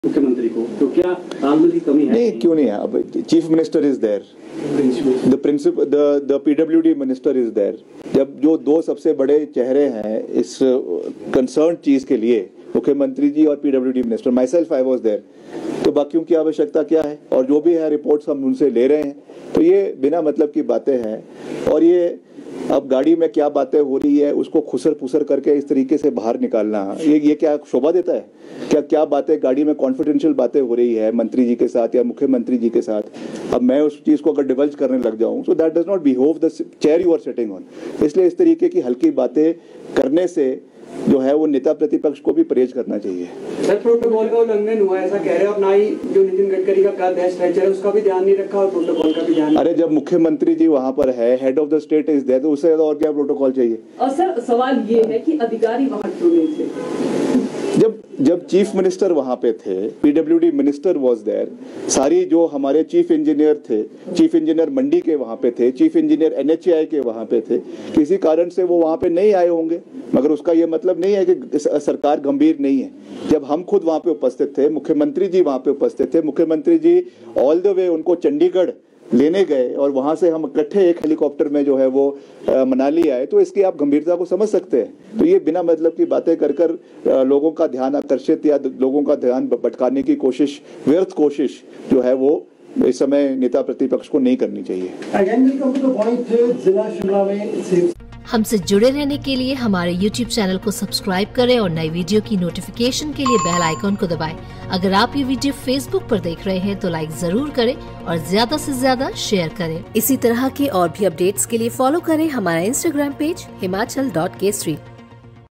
विपक्ष के सवालों पर विक्रमादित्य का जवाब: जब नितिन गड़करी के साथ मैं और CM थे तो अधिकारियों
विपक्ष के सवालों पर विक्रमादित्य का जवाब: जब नितिन गड़करी के साथ मैं और CM थे तो अधिकारियों की क्या जरूरत ?